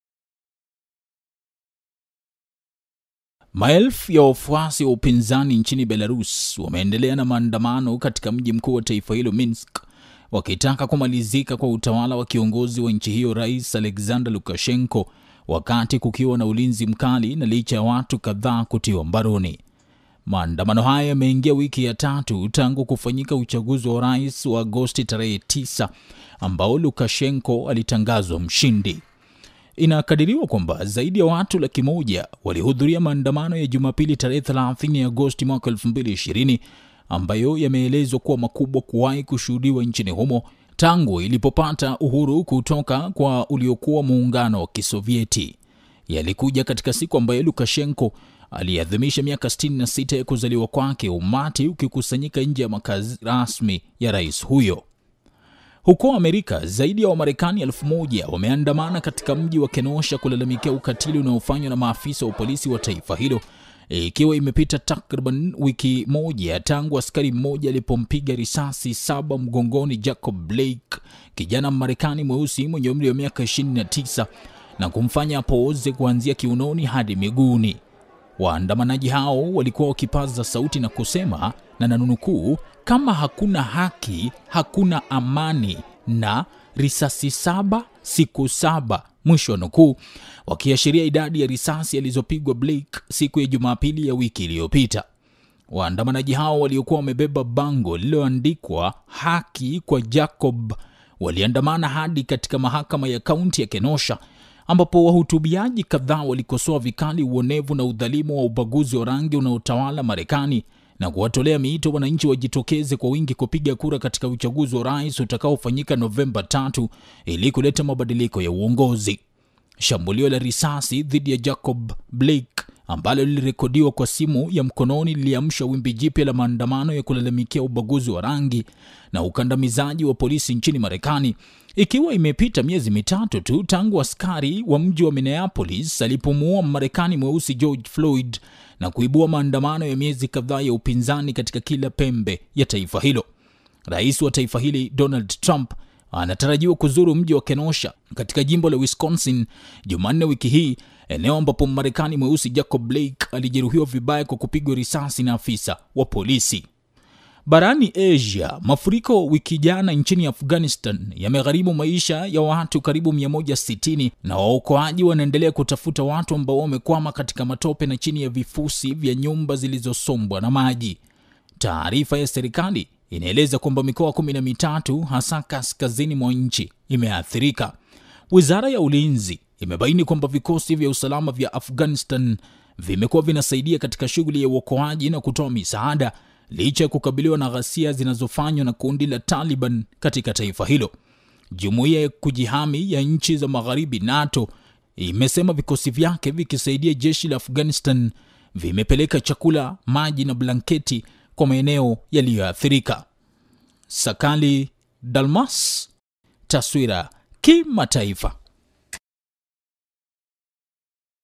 TASWIRA KIMATAIFA : Maandamano ya Upinzani yaendelea Belarus, 160 wafariki kwa mafuriko Afghanistan
Wafuasi Wa Upinzani Nchini Belarus Wameendelea Na Maandamano Mjini Minsk Wakitaka Kumalizika Kwa Utawala Wa Rais Alexander Lukashenko Wakati Kukiwa Na Ulinzi Mkali Na Licha Ya Watu Kadhaa Kukamatwa.

Maandamano Hayo Yameingia Wiki Ya Tatu Tangu Kufanyika Uchaguzi Wa Rais Wa Agosti 9 Ambao Lukashenko Alitangazwa Mshindi.

Huku Mafuriko Wiki Jana Nchini Afghanistan Yakigharimu Maisha Ya Karibu Watu 160 Na Waokoaji Wanaendelea Kutafuta Watu Ambao Wamekwama Kwenye Matope Na Chini Ya Vifusi Vya Nyumba Zilizosombwa Na Maji.

Taarifa Ya Serikali Inaeleza Kuwa Mikoa Kumi Na Mitatu, Hasa Kaskazini Mwa Nchi, Imeathirika